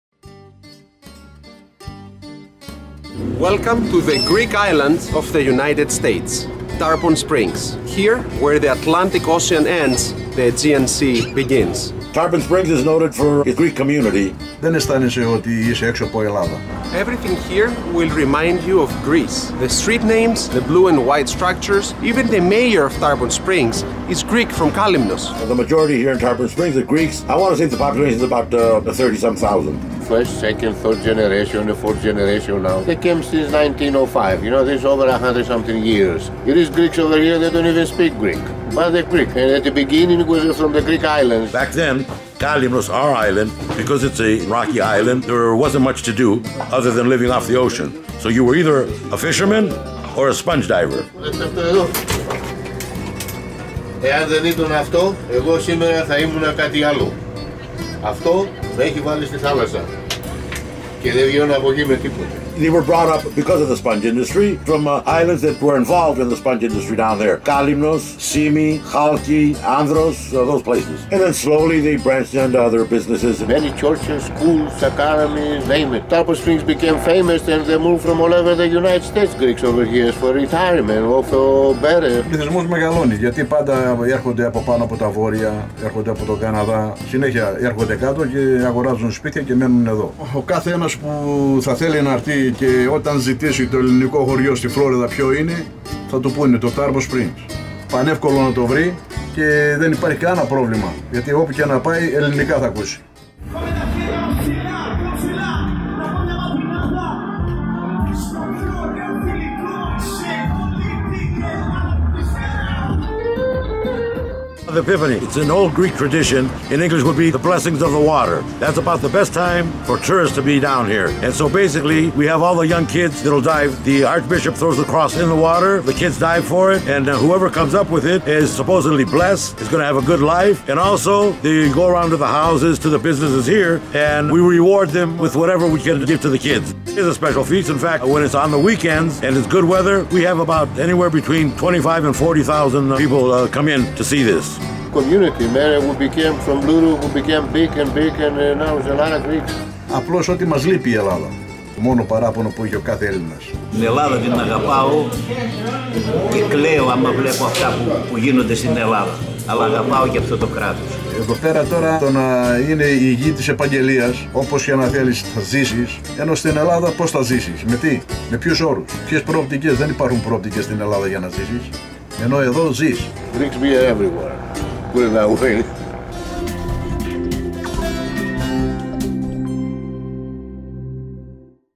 Tarpon Springs: The "Greek Island" of the United States
- Where the Atlantic Ocean ends, the Aegean sea begins. Discover Tarpon Springs, the most Greek village in the United States, created thanks to a few sponge divers that arrived in Florida in the early 1900's and thriving till today.